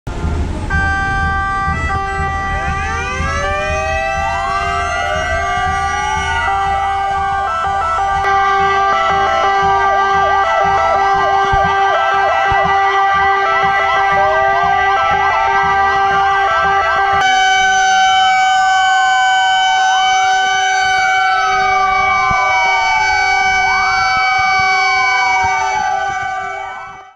Inaugurazione BIASEV 222 Croce Bianca Milano Sez. Seveso
Nonostante il maltempo, bellissima inauguarazione della nuova ambulanza della Croce Bianca Milano sez. Seveso, la 222, in occasione dei festeggiamenti per il loro 40°!